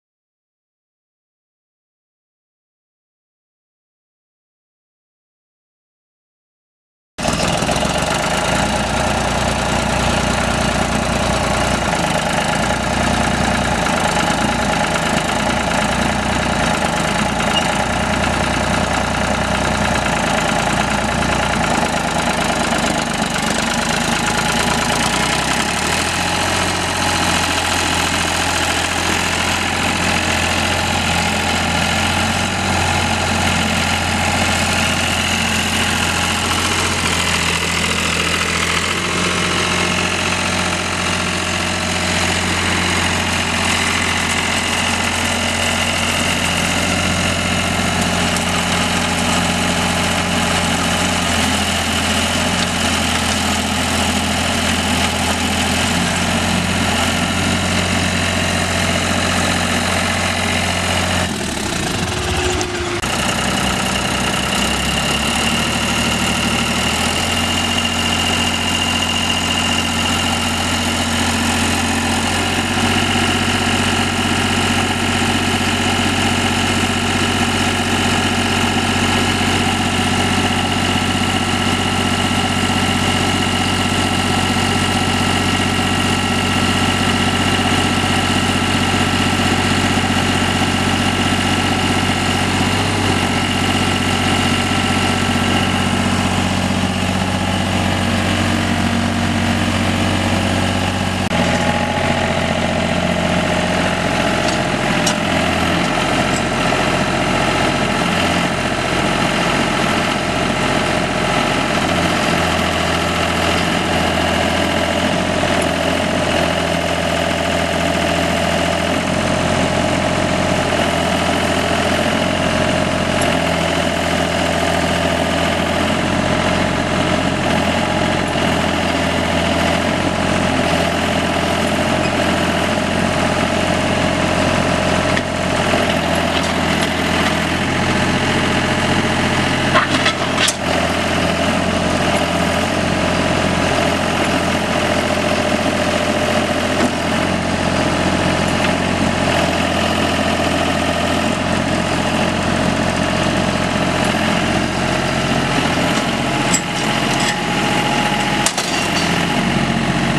1997 Ditch Witch 3700DD trencher for sale | sold at auction September 10, 2015
1997 Ditch Witch 3700DD trencher
932 hours on meter
Deutz three cylinder diesel engine
Hydrostatic transmission
H312 trencher attachment
6&#039; maximum digging depth
Side shift
A222 backhoe attachment
18"W
Serial 7P2980
64"W back fill blade
Hydraulic bore drive included
Rear wheel weights
29 x 12.50-15 tires
Serial 3P1054

It was located in Gardner, KS, and we sold it to the highest bidder for $8,470.